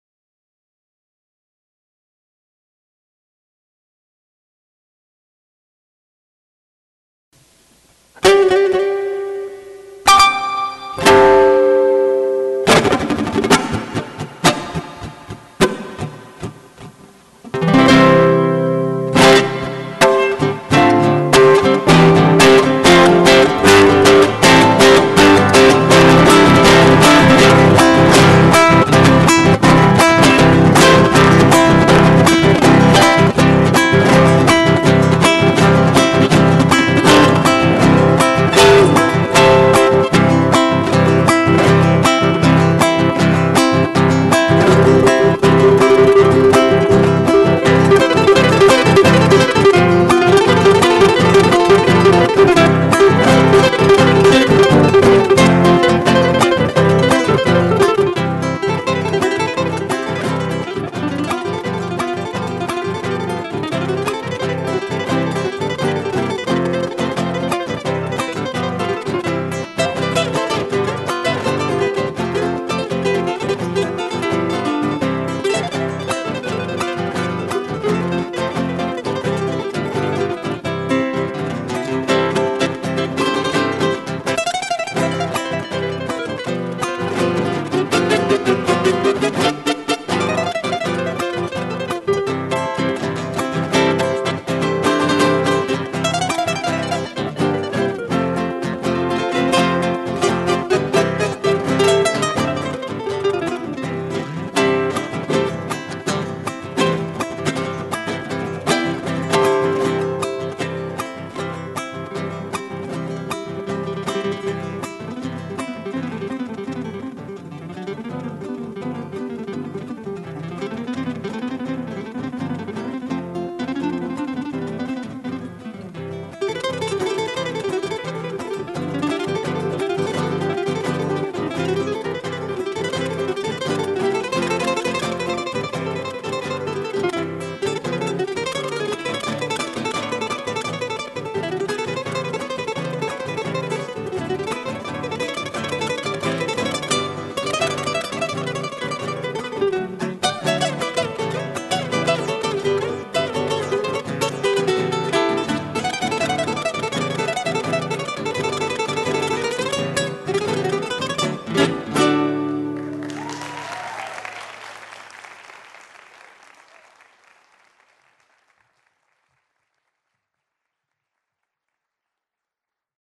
Panchi Duarte live at the Ibero-American Guitar Festival
Panchi Duarte (Paraguay) performing at the 4th Annual Ibero-American Guitar Festival in Washington, DC assisted by Cesar Aquino. The festival took place June 2010 at the Smithsonian National Museum of the American Indian. The Ibero-American Guitar Festival is presented by the Ibero-American Cultural Attaches Association in Washington D.C.